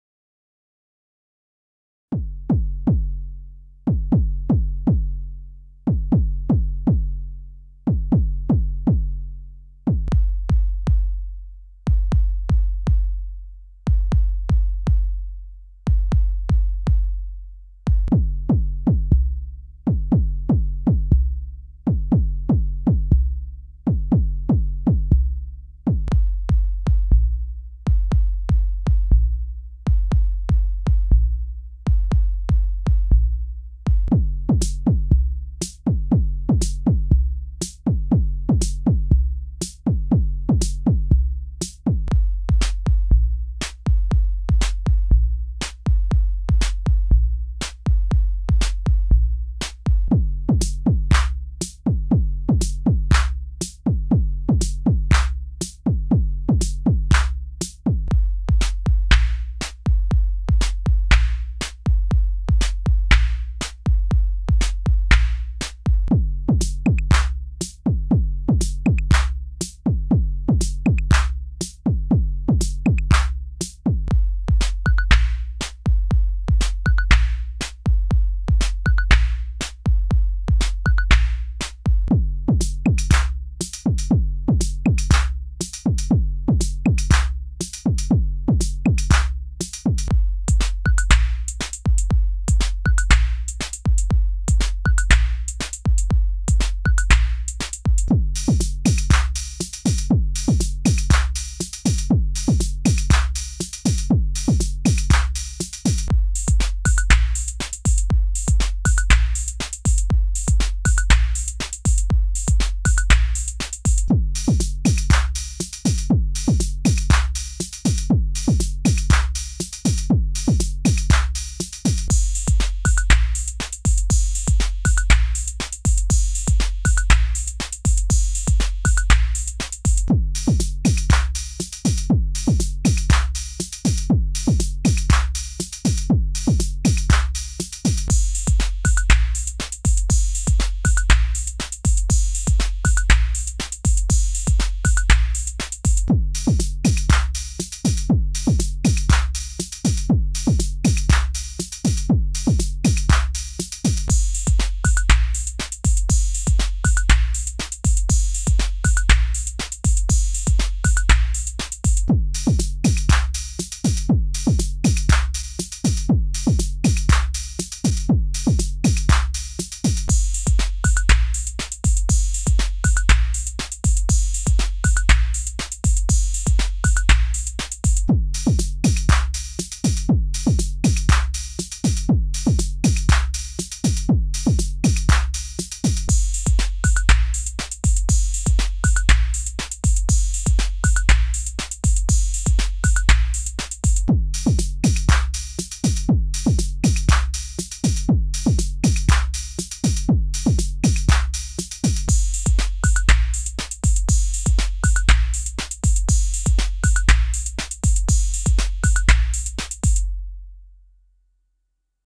Arturia Drumbrute VS MFB Tanzbar
Comparison of the Drumbrute and Tanzbar.

MIDI clock is sent from the Drumbrute to the Tanzbar.